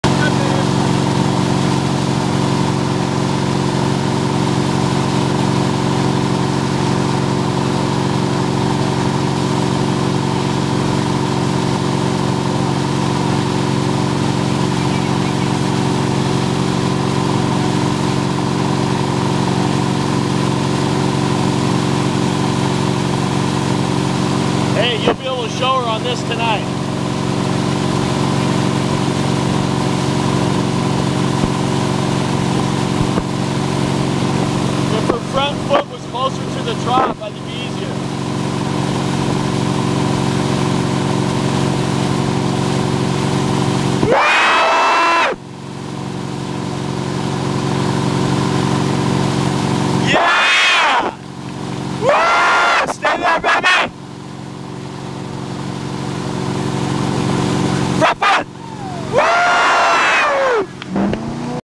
Maria Tortilla drops the rope
My Maria drops the rope and wakesurfs!